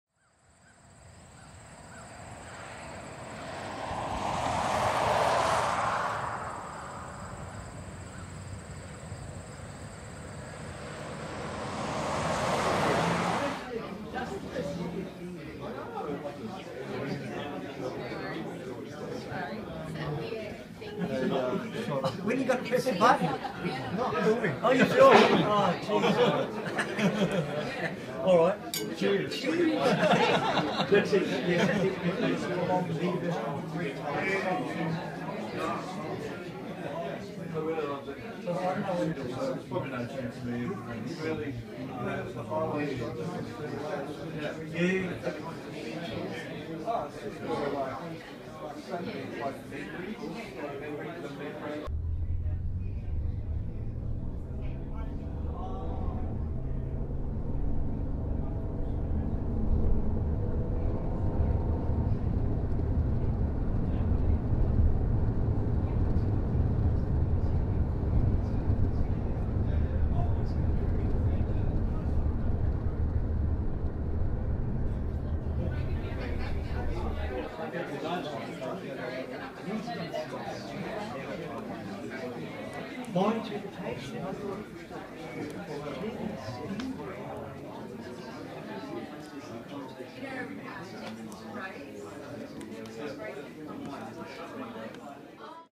Bourne In - Dark Solace, Exhibition at the Quirk Gallery
An exhibition of photos by Richard Szymczuk
25 May to 6 July 2014, Tue - Sun
Quirk Space
85 Pakington Street, Geelong West.